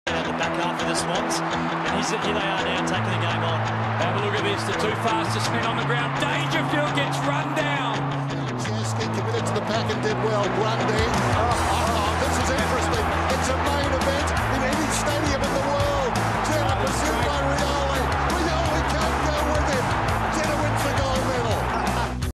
Lewis Jetta: On The Rise - AFL
With a premiership medal under his belt, Swans livewire Lewis Jetta will only improve further in 2013.